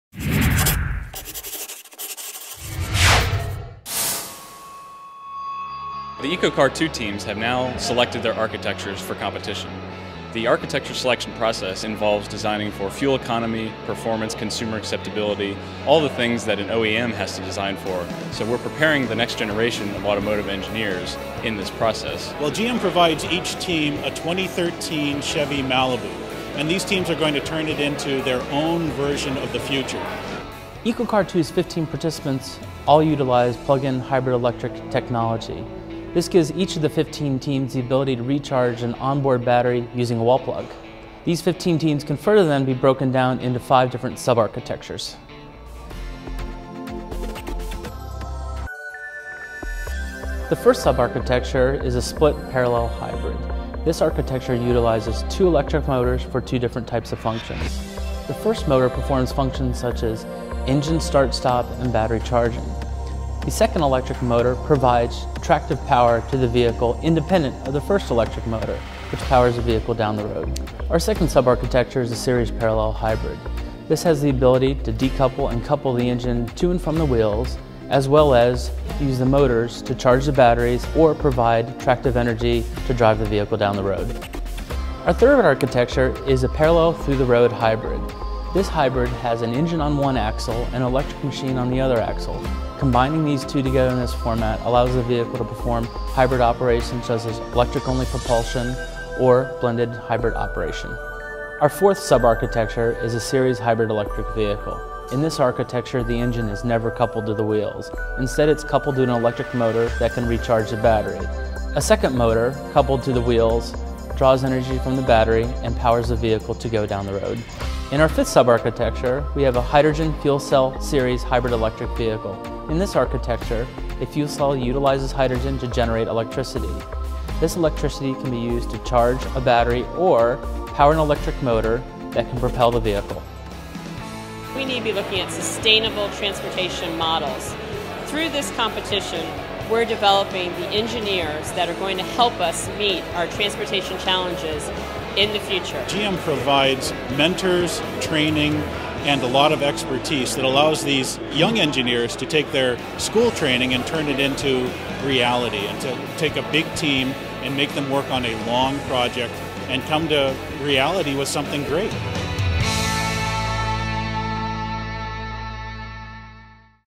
EcoCAR 2 Vehicle Architecture Announcement
EcoCAR 2 announces the student vehicle architectures based on the 2013 Chevy Malibu platform